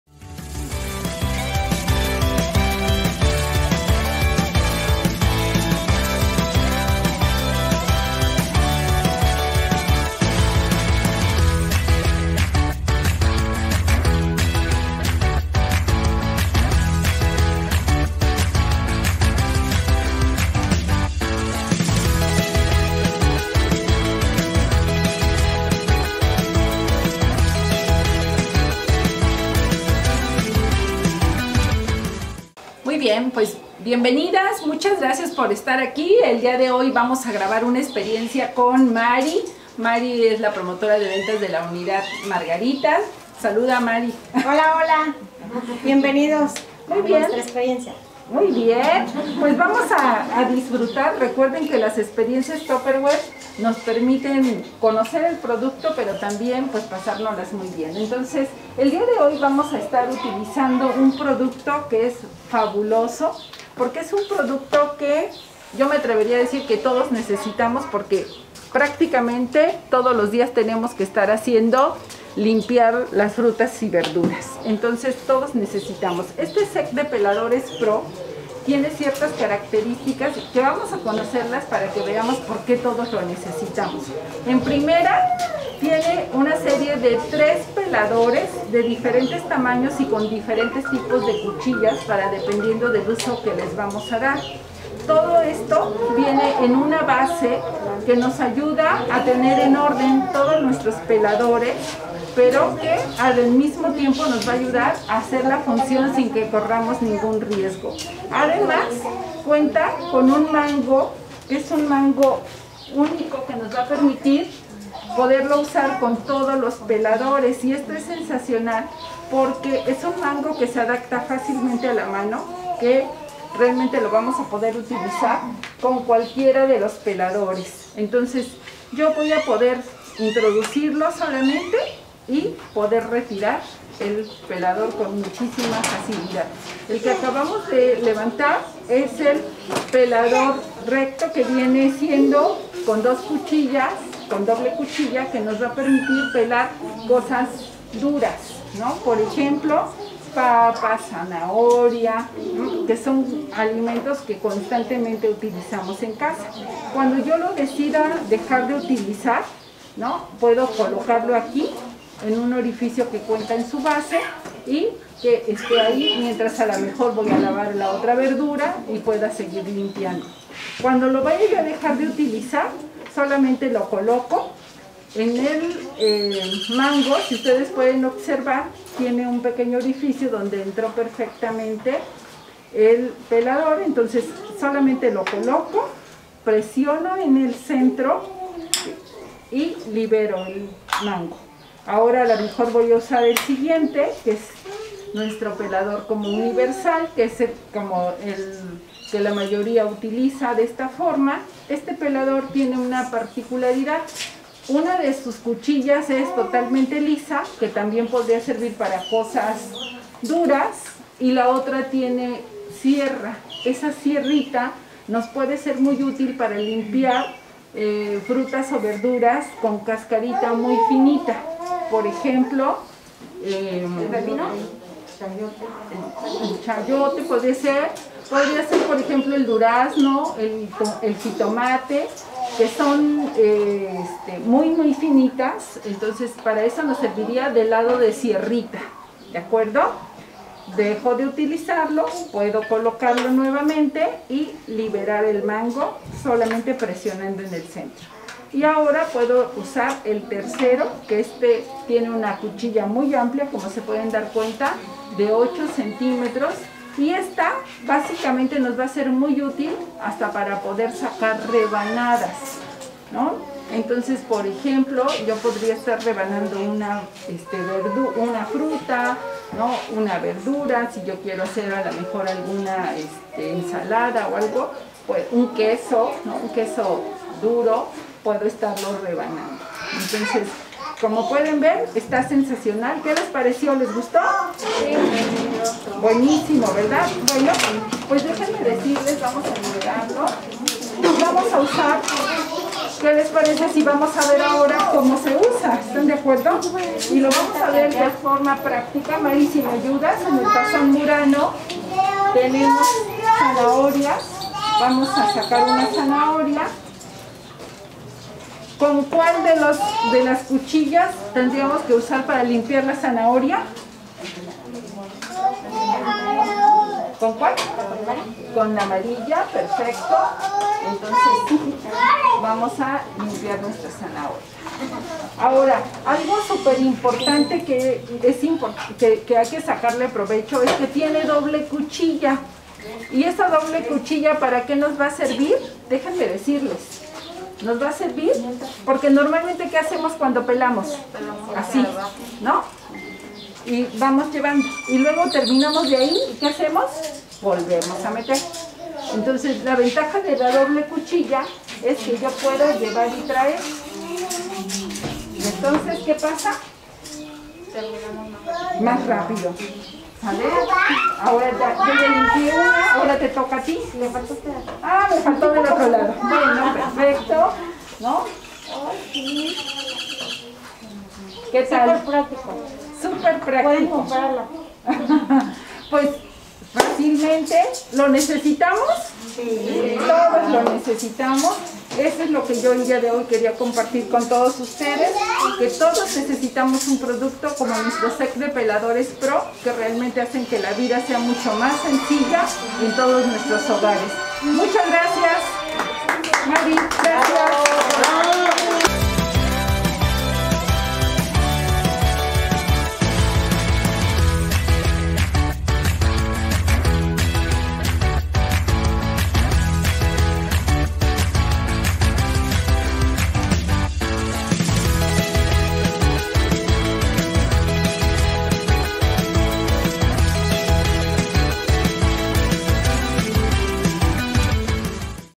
Set de Peladores ft. Unidad Margarita | Experiencias Tupperware | Tupperware Reyna
En esta ocasión acompañamos a la unidad margarita, y decidimos grabar una experiencia un poco diferente, pero sin duda interesante.
Esperemos les guste mucho